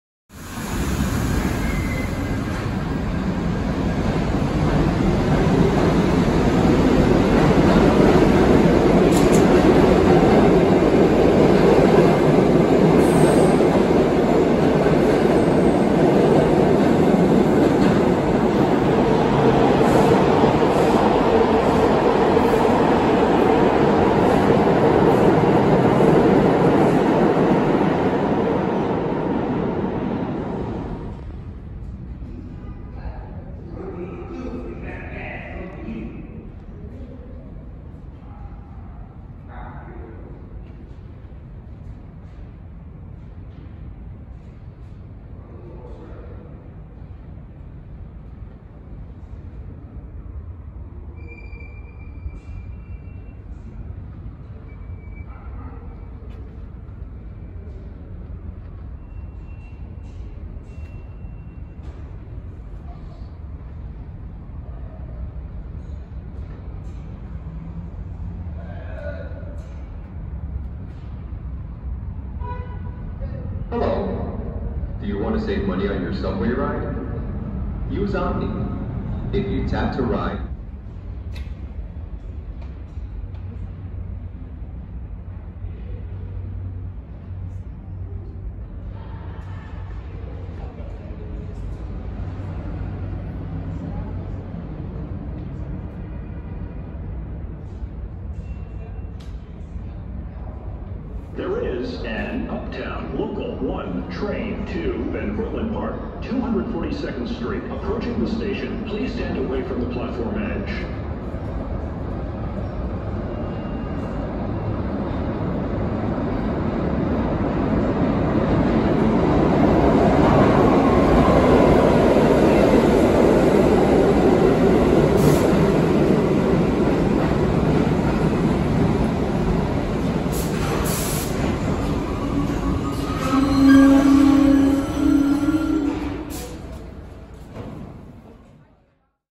Tactile & Braille ADA 1 Train Line Map at 23rd St (Seventh Ave Line)
Here are several videos taken on April 8, 2022, featuring the 23rd St station on the 1 train. On the northbound platform of this station, there is an interesting line map of the northbound 1 train with tactile text and accompanying Braille, likely for ADA passengers. No other line maps of this format exist elsewhere at the time this video was taken or uploaded (May 2022).